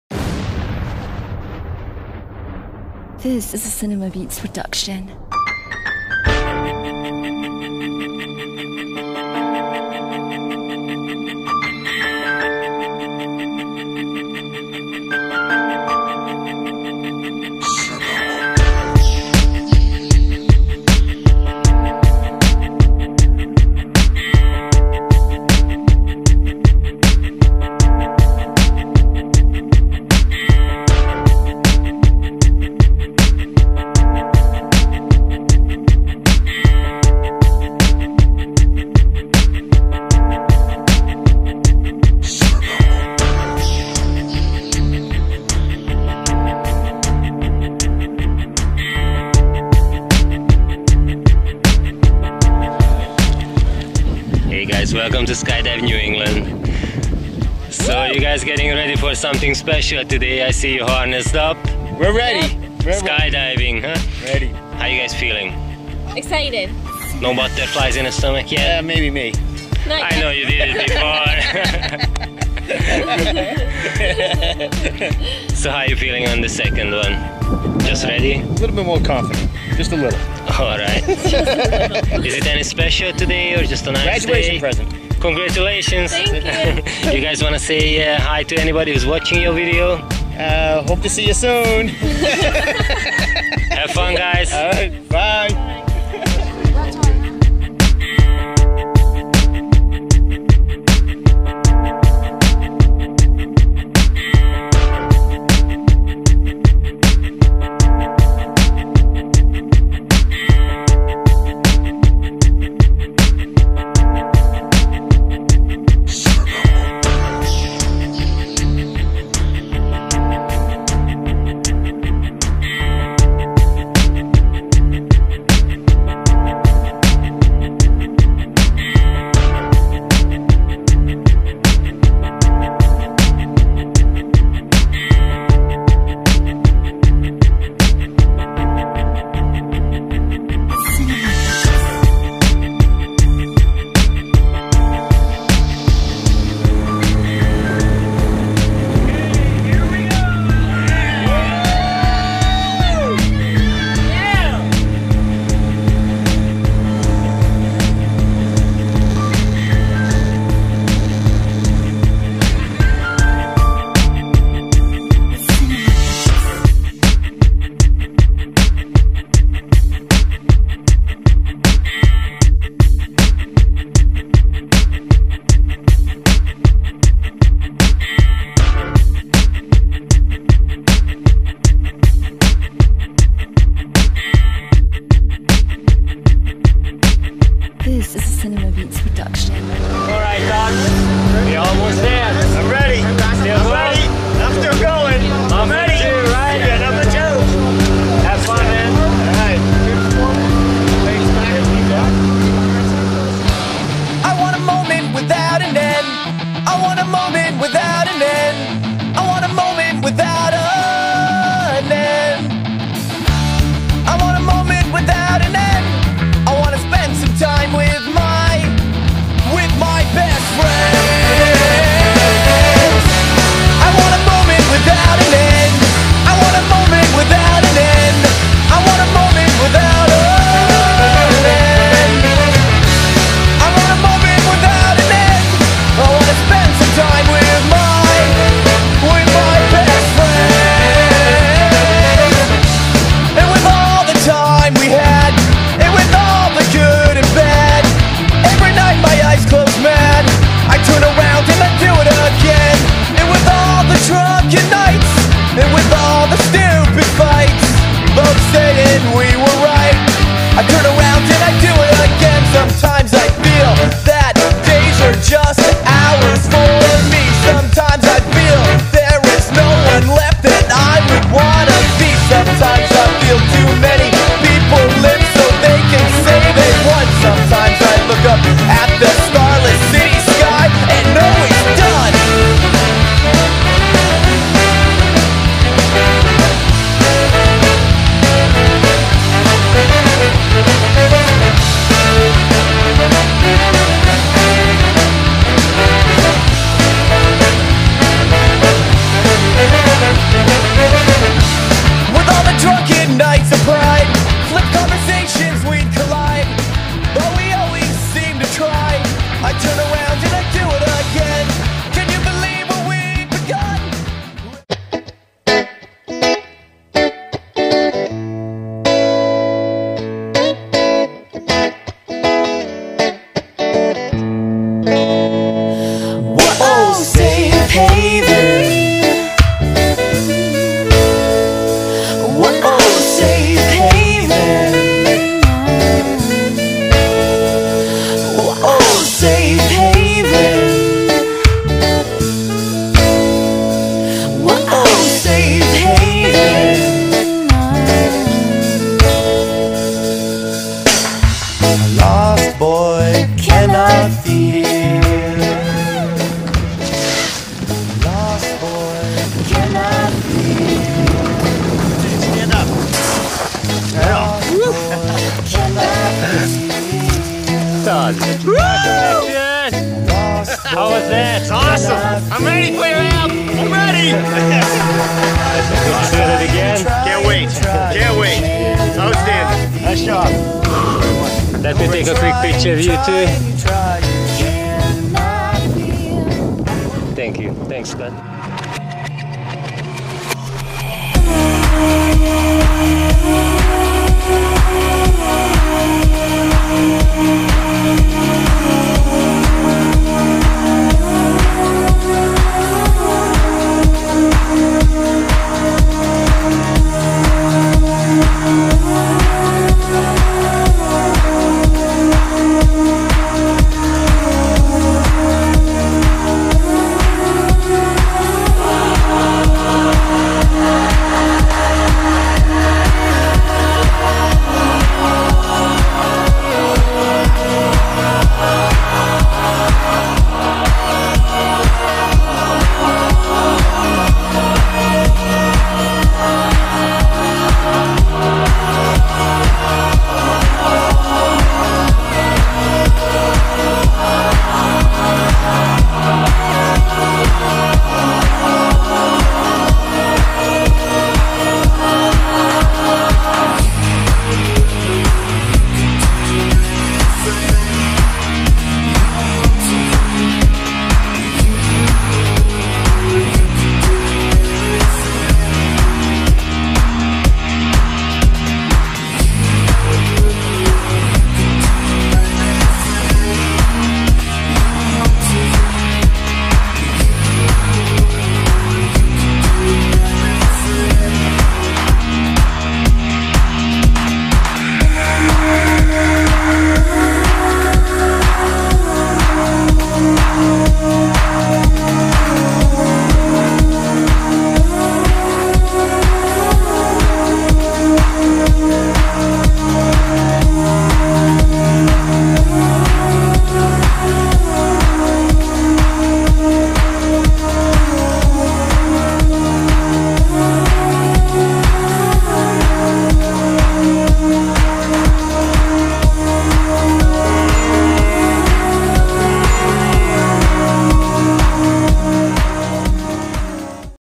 Todd - Jumps at Skydive New England
Todd - makes a jump at Skydive New England